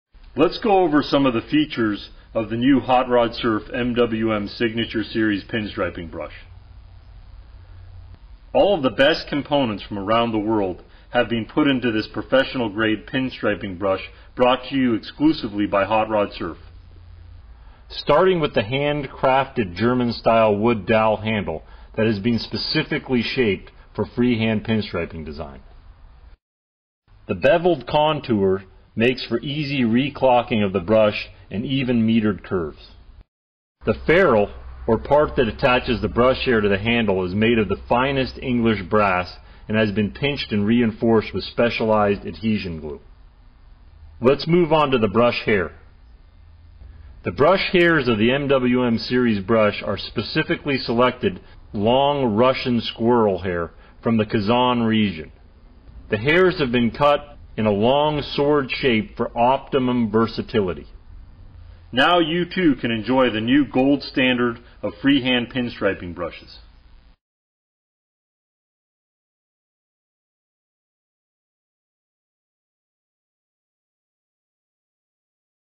Hot Rod Surf Pinstriping Brush by MWM
HOT ROD SURF brings you the ultimate pinstriping brush for automotive freehand pinstriping. This special brush is the first in our exclusive line of professional signature series brushes for classic 1950's style pinstriping design applications.  The MWM Signature Series Brush™ is made with the finest materials from around the world starting with a custom shaved German style wood handle, pinched and glued with an English brass ferrule, and loaded with specially selected exotic Russian squirrel hair.  Each brush is hand stained, printed, sealed and assembled with the finest brush workmanship on the planet. This special brush is the new gold standard of pinstriping brushes for freehand pinstriping design.